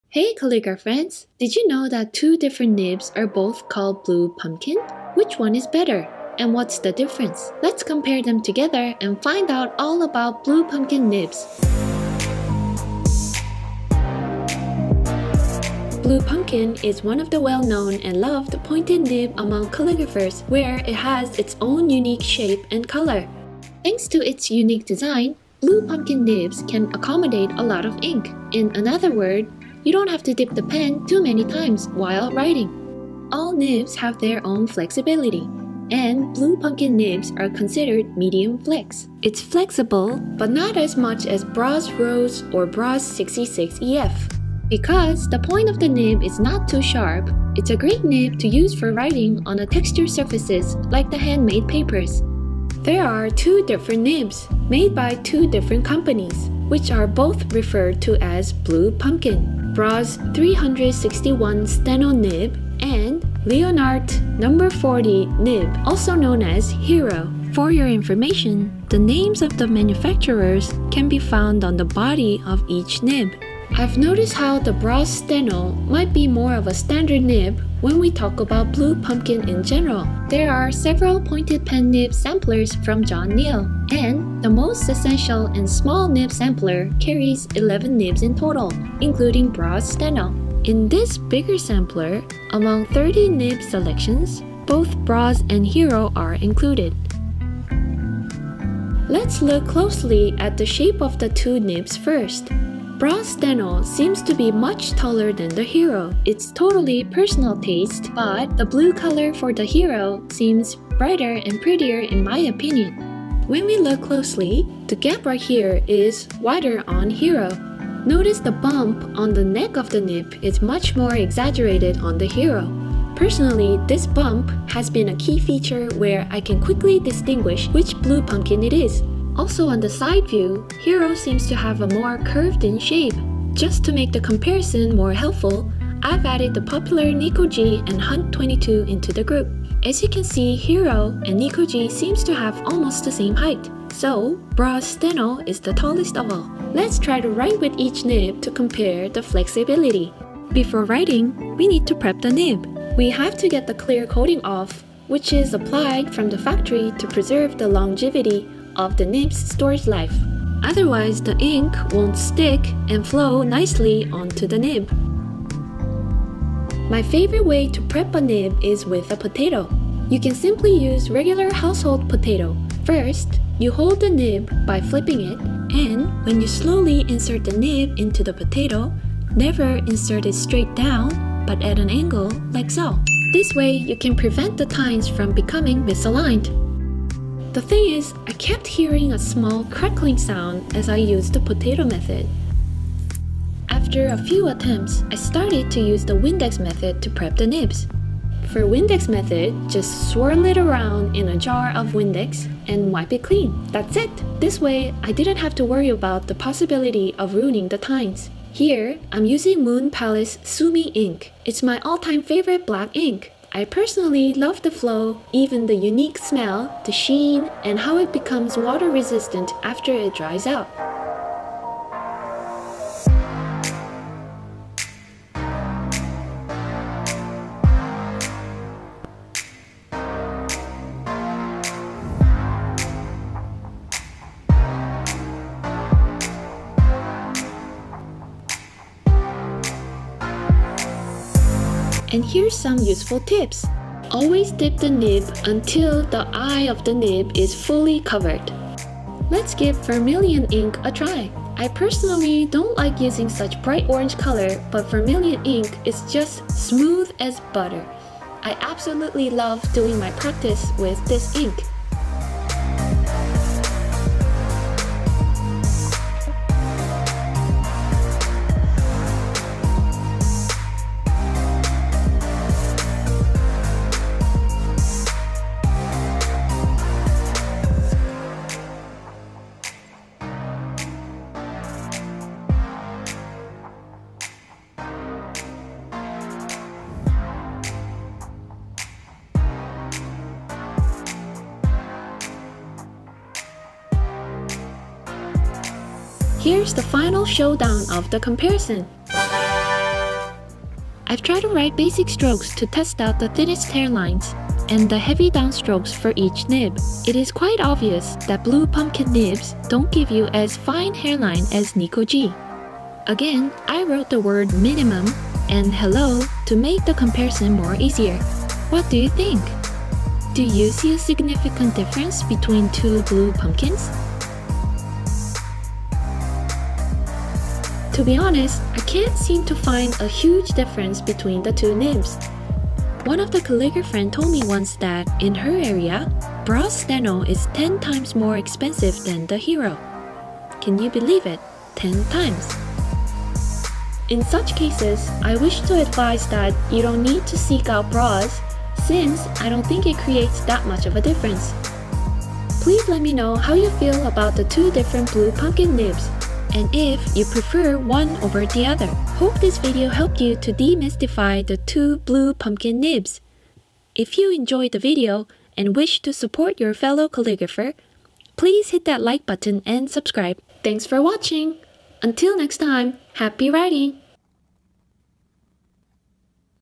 Blue Pumpkin Nibs Comparison - Brause 361 Steno VS Leonardt Hiro 40
Blue Pumpkin is one of the well known and loved pointed nib among calligraphers. Today’s video has been inspired by one of the calligrafriends’ curiosity as to the difference between the two nibs, which are both called Blue Pumpkin. Which one is better and what’s the difference? Let’s compare them together and find out all about Blue Pumpkin Nibs!